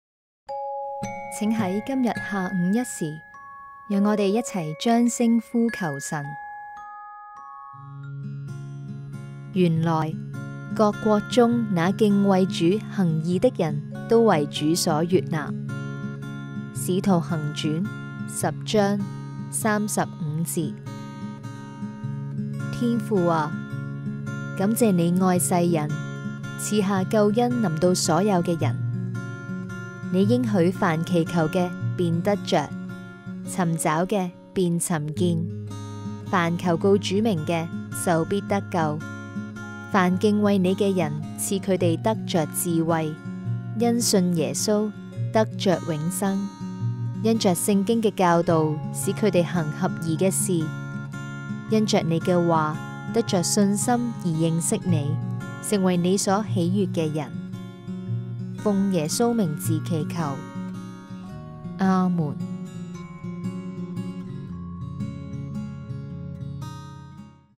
一源讀經 2020/06/22
"原來, 各國中那敬畏主、行義的人都為主所悅納。" 使徒行傳10:35
天父啊！感謝祢愛世人，賜下救恩臨到所有的人。祢應許凡祈求的便得著，尋找的便尋見，凡求告主名的就必得救。凡敬畏祢的人，賜他們得著智慧，因信耶穌得著永生；因著聖經的教導使他們行合宜的事；因著祢的話，得著信心而認識祢，成為祢所喜悅的人。奉耶穌名字祈求，阿們！